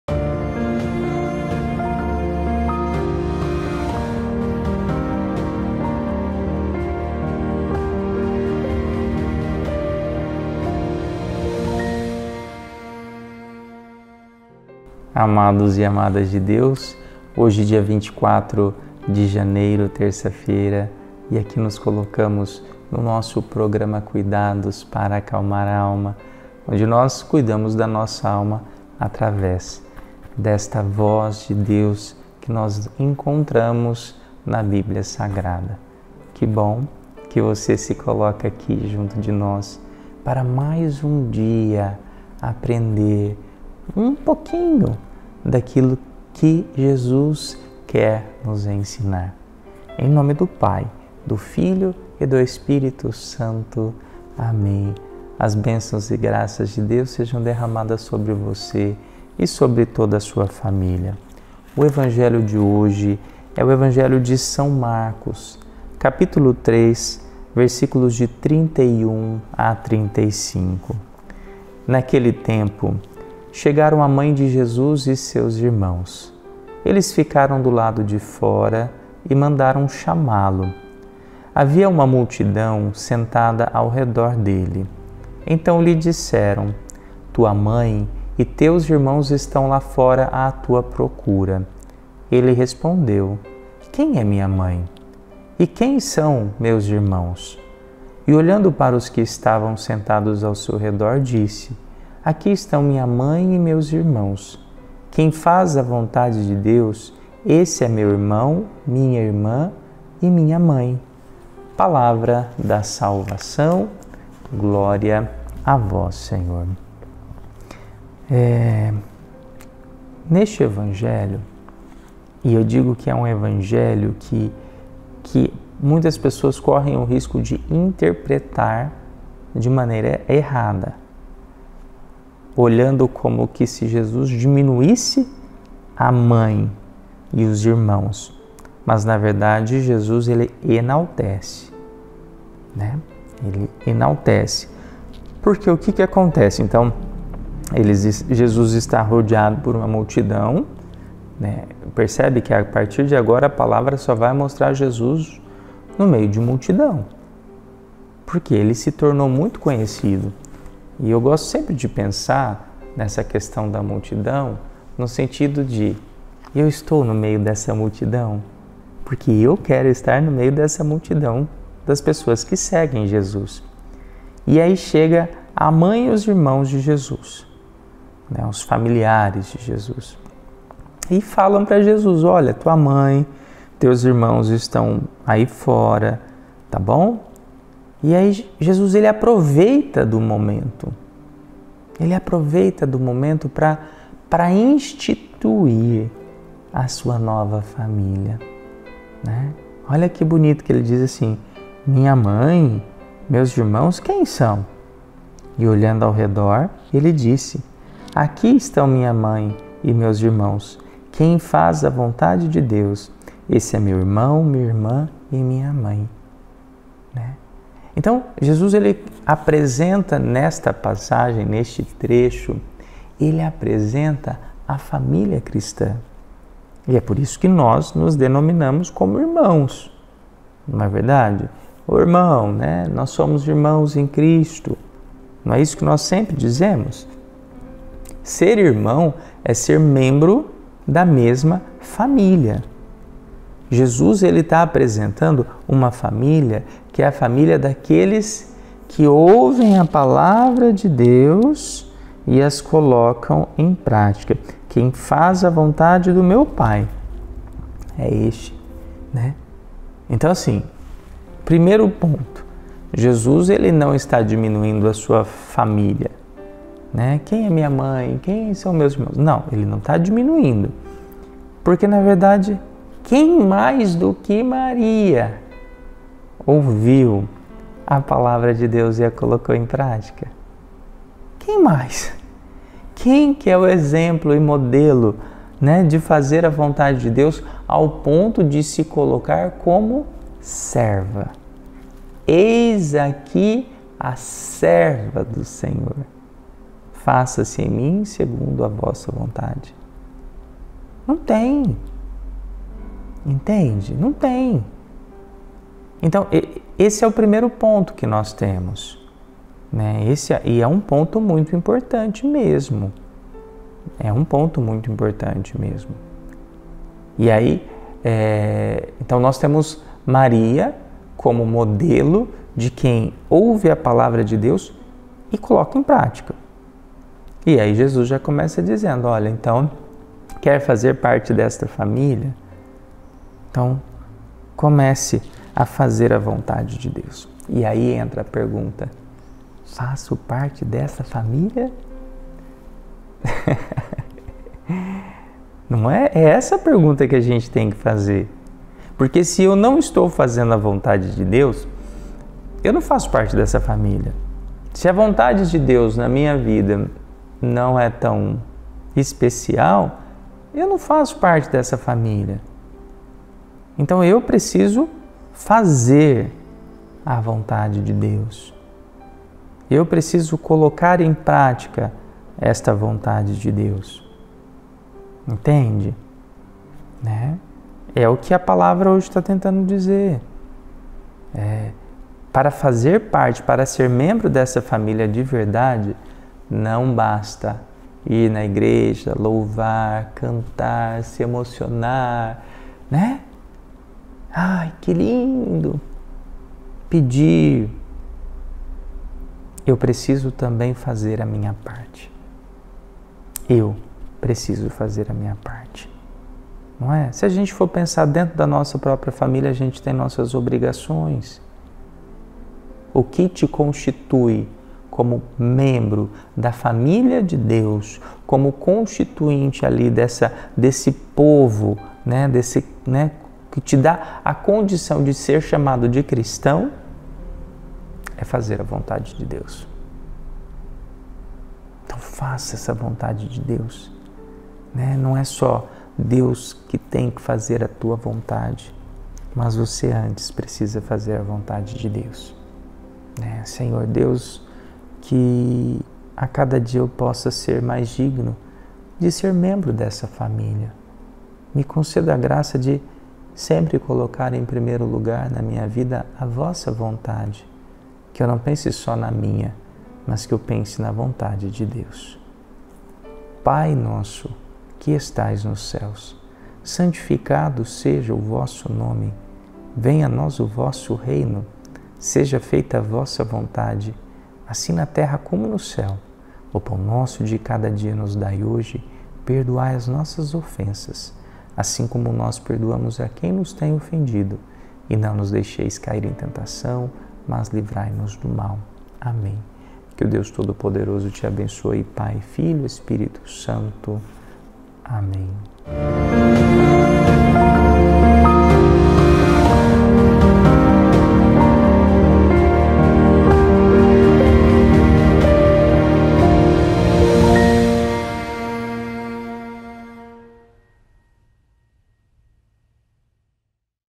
Homilia Diária Terça-Feira São Francisco de Sales (Bispo e Doutor da Igreja)
A proximidade e a comunhão com Jesus Cristo se efetivam não por força de gestos rituais ou de vínculos humanos, mas pelo comprimento da vontade de Deus.